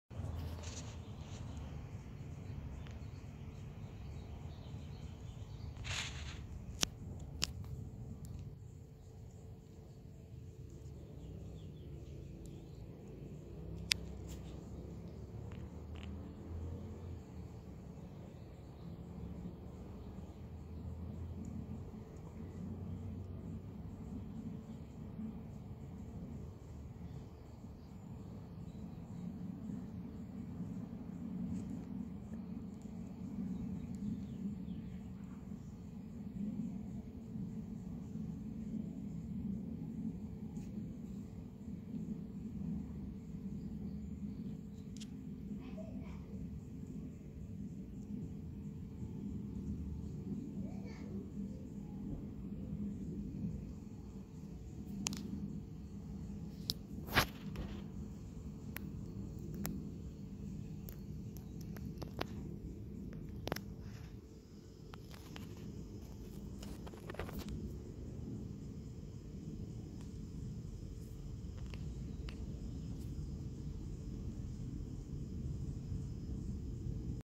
Iron beetle drinking water.
Iron beetle drinking water in 4K close up.